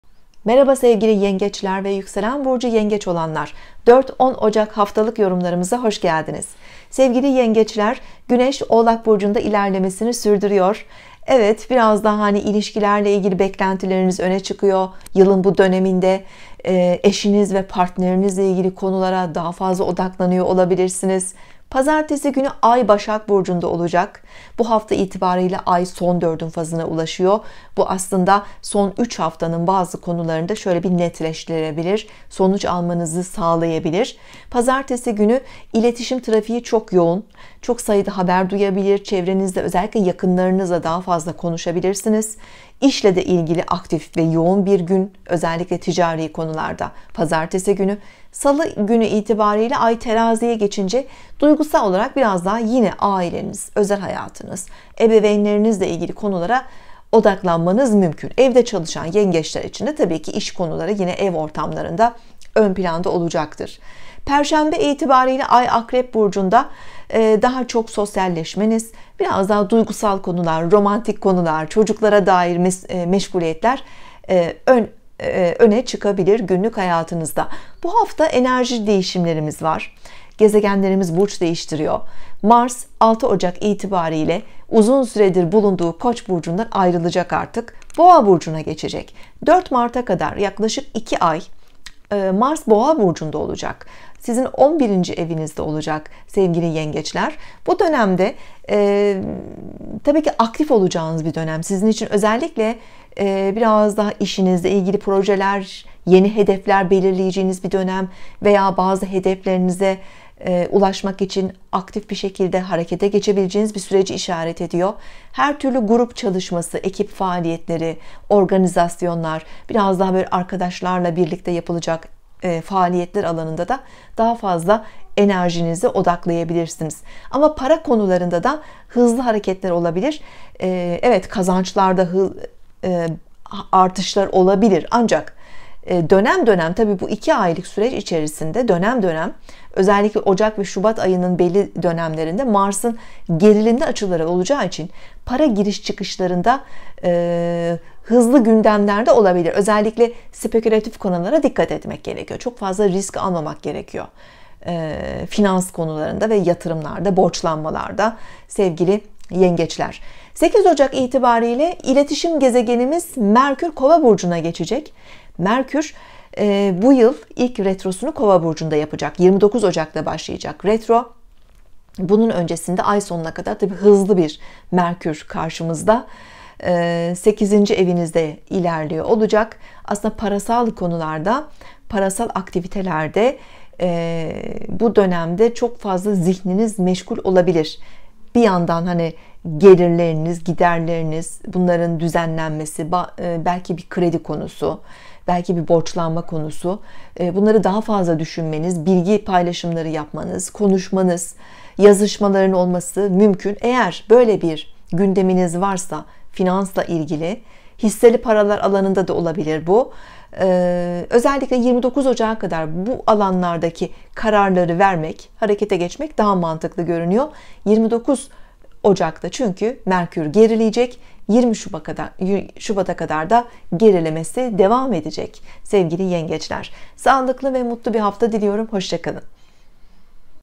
Haftalık Burç Yorumları Yengeç Burcu 4 Ocak 2021 Astroloji
3 - 10 ocak 2021 Haftalık Burç Yorumları (yengeç ve yükselen yengeç burcu)
yengeç ve yükselen yengeç burcu 3 - 10 ocak 2021 haftası haftalık sağlık, iş, kariyer haftalık burç yorumları (Astroloji)